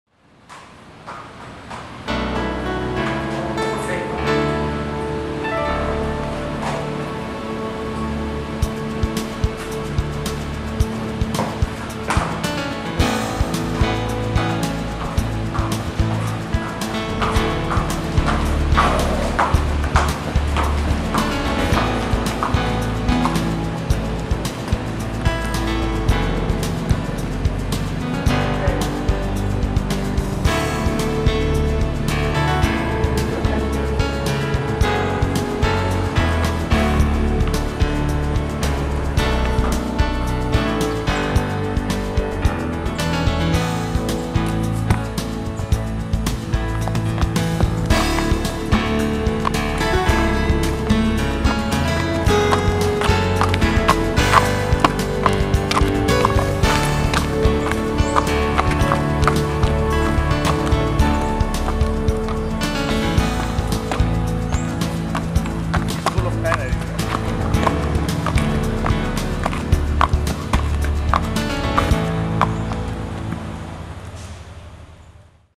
The Green Monkey - $16M Thoroughbred in Florida 2009
Here is The Green Monkey at his home in Florida, standing stud duty after costing a world record $16 million at the 2006 Fasig Tipton sale. We filmed this in August 2009